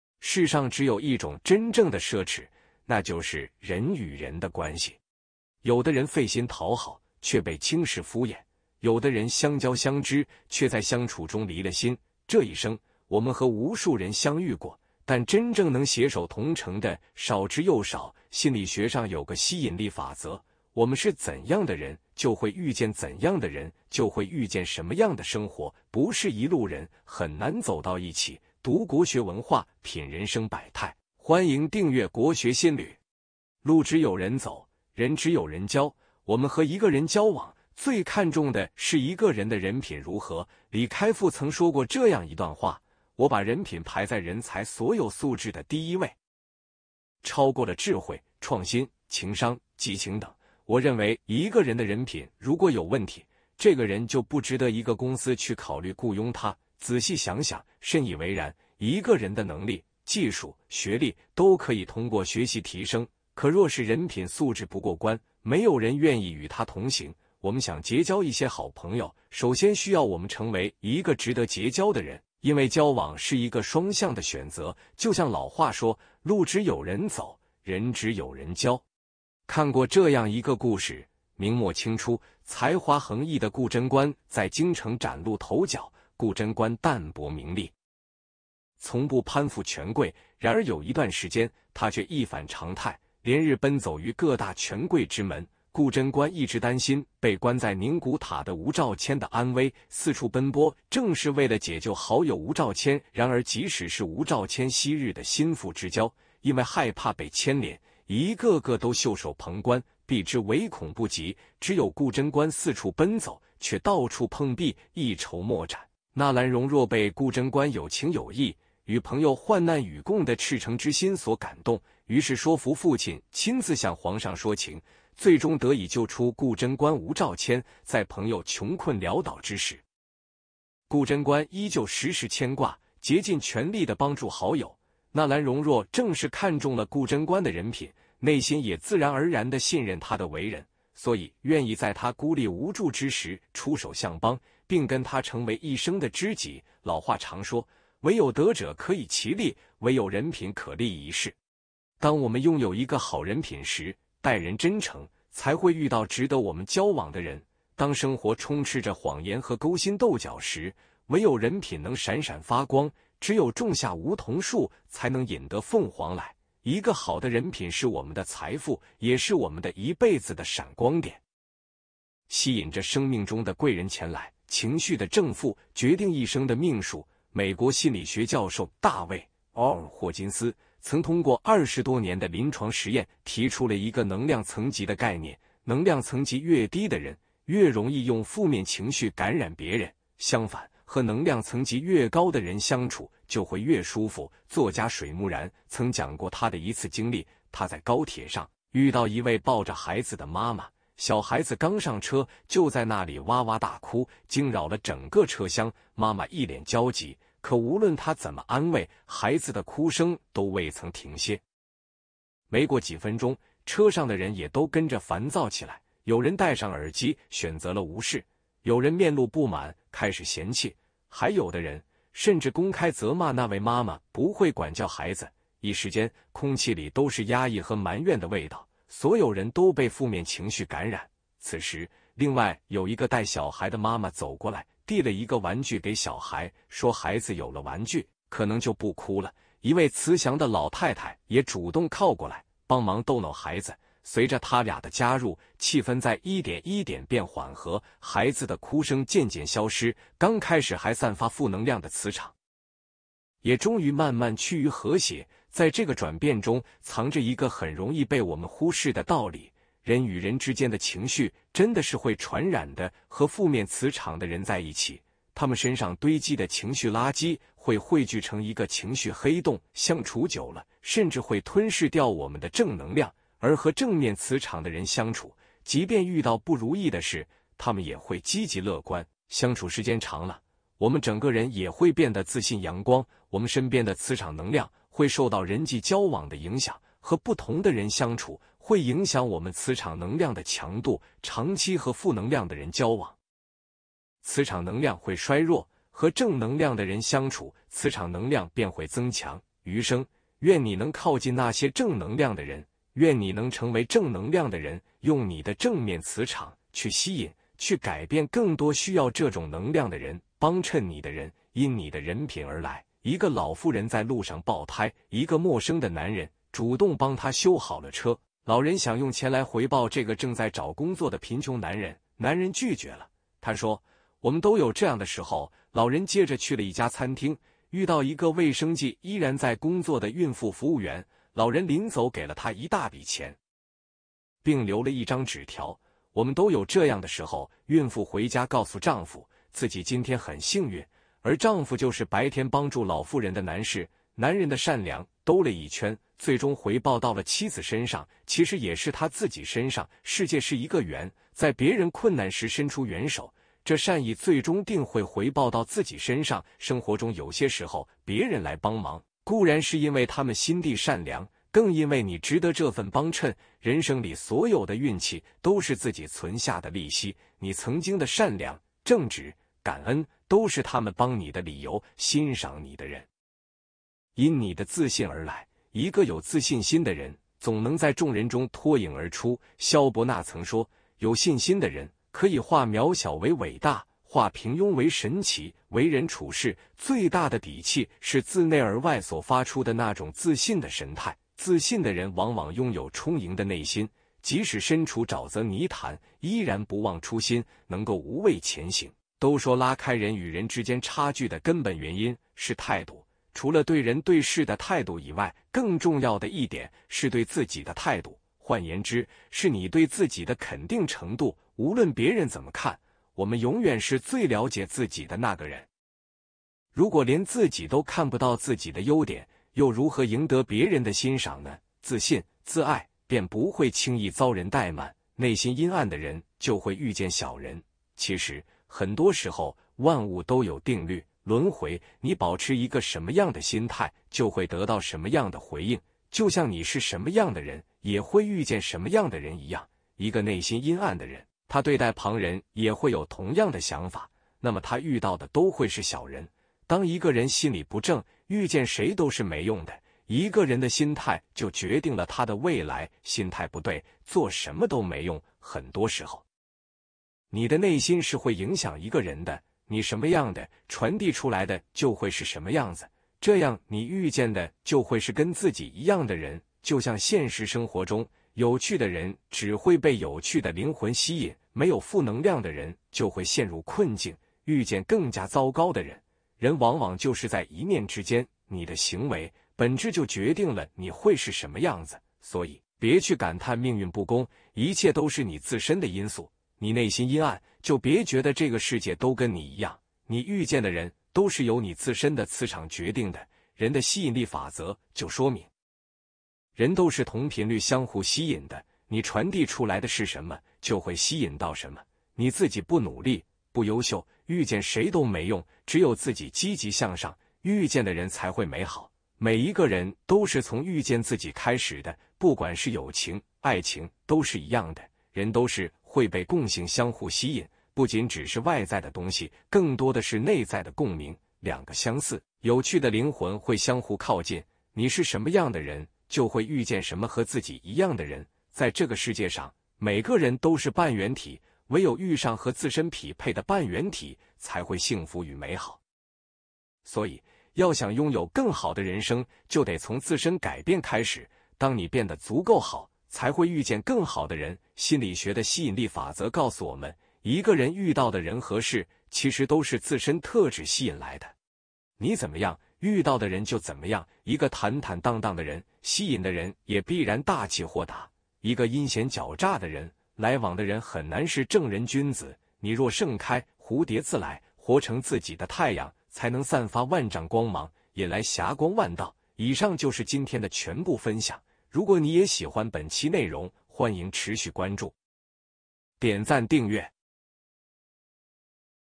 一個人願意與你相處，不是因為聊得來，而是因為「磁場定律」！【國學心旅】為人處世
「世上只有一種真正的奢侈，那就是人與人的關系。」有的人費心討好，卻被輕視敷衍；有的人相交相知，卻在相處中離了心。這一生，我們和無數人相遇過，但真正能攜手同程的，少之又少。心理學上有個吸引力法則：我們是怎樣的人，就會遇見怎樣的人，就會遇見什麽樣的生活，不是一路人，很難走到一起。為人處世